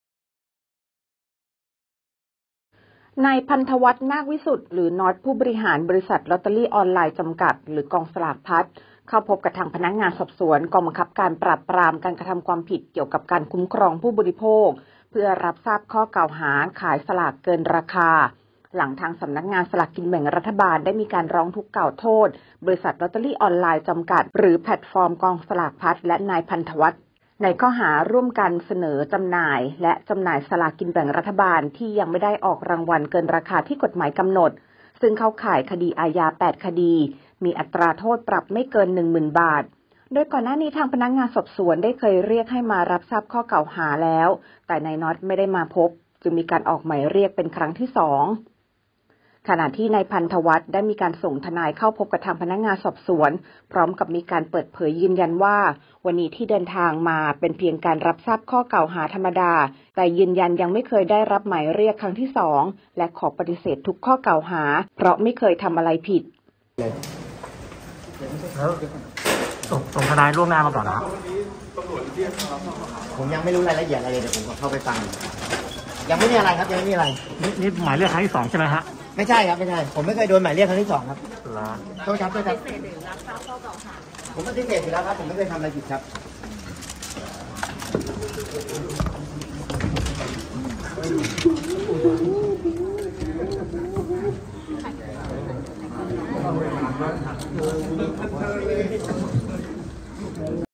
"นอท" กองสลากพลัส เข้าพบ ปคบ. รับข้อกล่าวหาขายสลากเกินราคา
นายพันธ์ธวัช นาควิสุทธิ์ หรือ นอท กองสลากพลัส พร้อมทนายความ เข้าพบพนักงานสอบสอบ กองบังคับปราบปรามการกระทำความผิดเกี่ยวกับการคุ้มครองผู้บริโภค หรือ ปคบ. เพื่อรับทราบข้อหาขายสลากเกินราคา หลังสำนักงานสลากกินแบ่งรัฐบาล ได้มอบนิติกรร้องทุกข์กล่าวโทษบริษัท ลอตเตอรี่ออนไลน์ จำกัด หรือ แพลตฟอร์มกองสลากพลัส และนายพันธ์ธวัช ในข้อหาร่วมกันร่วมกันเสนอจำหน่าย และจำหน่ายสลากกินแบ่งรัฐบาลที่ยังไม่ได้ออกรางวัลเกินราคาที่กฎหมายกำหนด ซึ่งเข้าข่ายคดีอาญา 8 คดี มีอัตราโทษปรับไม่เกิน 10,000 บาท  โดยก่อนหน้านี้พนักงานสอบสวน เคยเรียกให้เข้ามารับทราบข้อกล่าวหาแต่ นอท ไม่ได้มาพบ จึงออกหมายเรียกรอบที่ 2

ขณะที่ นายพันธ์ธวัช เปิดเผยเบื้องต้น ยืนยันว่า วันนี้ที่เดินทางมา ยังไม่ทราบรายละเอียดว่าเรื่องอะไร แต่ยืนยันว่าไม่เคยได้หมายเรียกครั้งที่ 2 และขอปฏิเสธทุกข้อกล่าวหา เพราะไม่เคยทำอะไรผิด